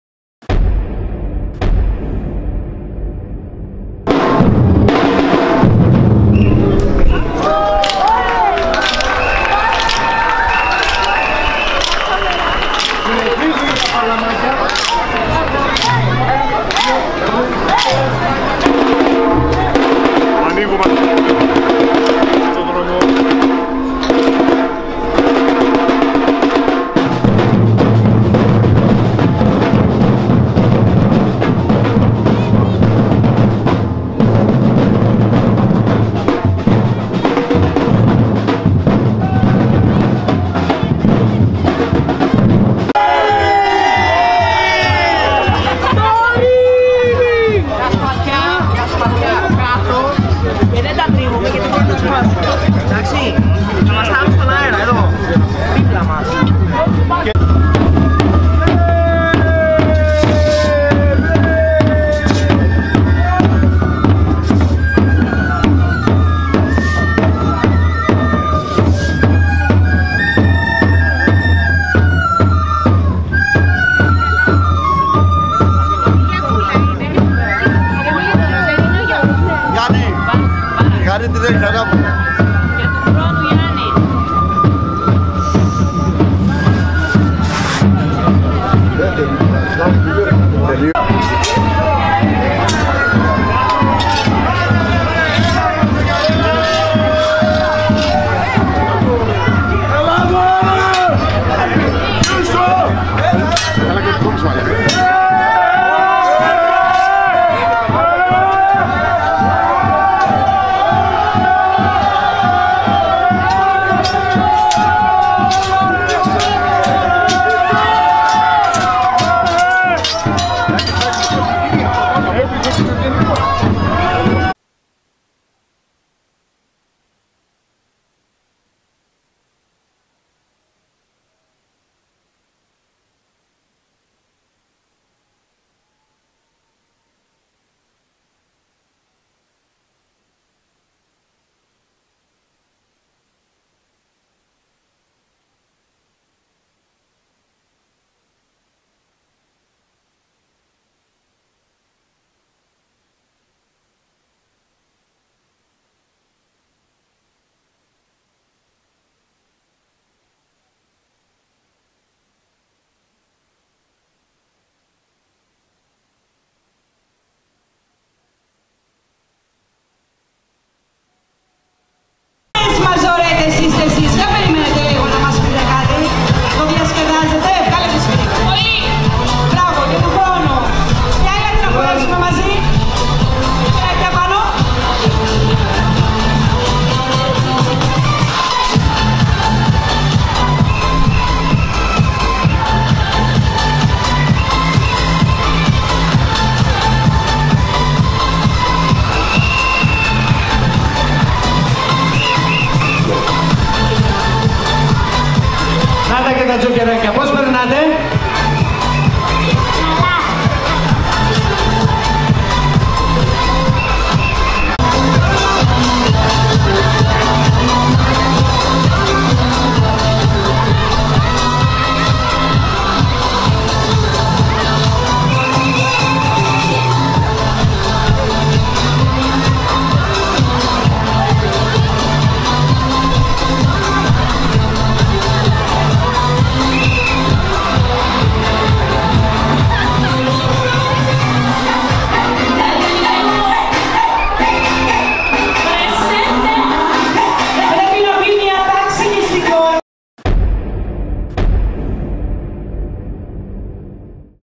Το κατέβασμα της Μόστρας στα Θυμιανά-18/02/18
Ελληνοτουρκική η φετινή Μόστρα αφού τουλάχιστον 1000 τούρκοι τουρίστες ξεκίνησαν απ΄ αυτή το τριήμερο της παραμονής τους.
Ήταν τόσο έντονη η παρουσία τους, που σωστά ο ΜΕΟΘ ξεκίνησε την περιγραφή με μετάφραση στην τουρκική γλώσσα, ενώ ξένοι και πάρα πολλοί ντόπιοι το γλέντησαν αφού οι ασύγκριτοι σ’  αυτά Θυμιανούσοι, τους συμπαρέσυραν στο ρυθμό του καρναβαλιού.